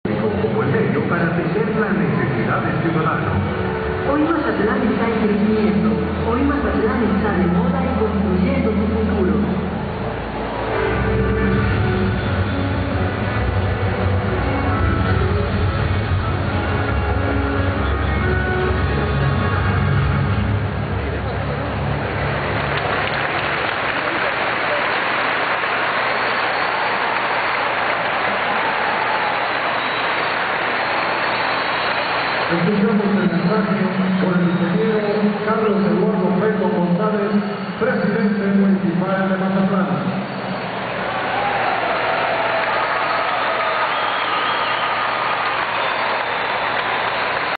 Carlos Felton 1er informe de gobierno mpal., cumplio con el pueblo
MAZATLAN,SINALOA ,EL ALCALDE CARLOS FELTON PRESIDENTE MUNICIPAL DE ESTE PUERTO EN SU PRIMER INFORME DE GOBIERNO MPAL.